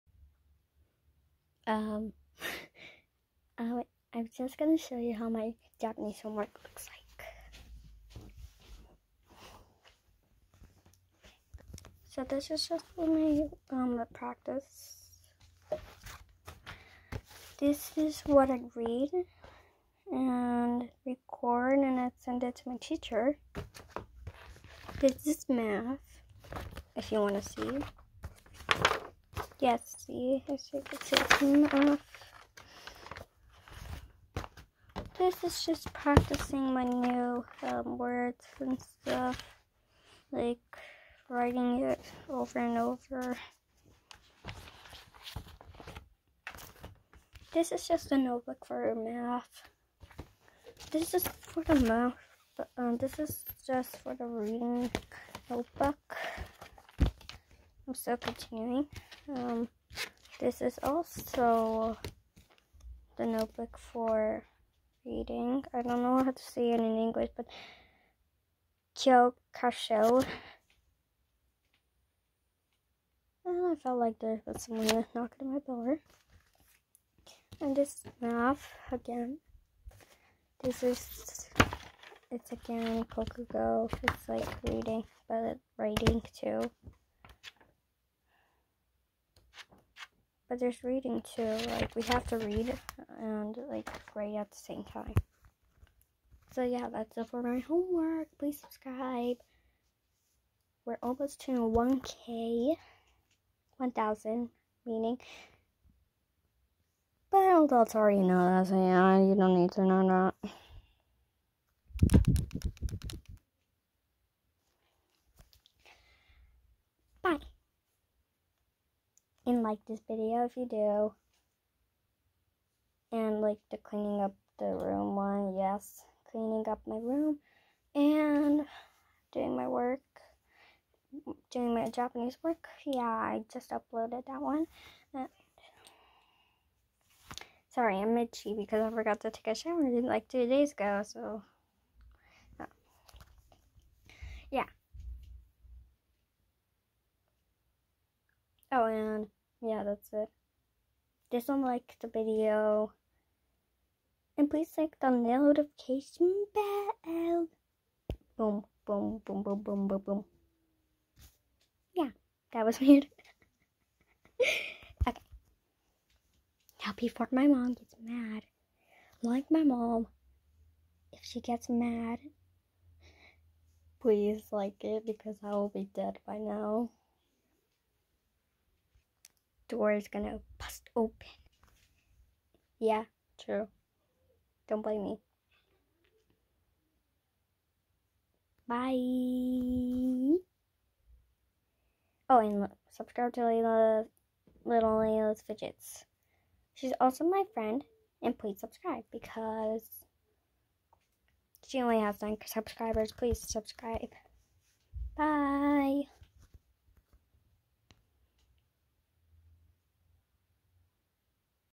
showing how my homework looks like(Japanese)
I just uploaded cleaning my room&Working on my Japanese work Part 2 is tomorrow.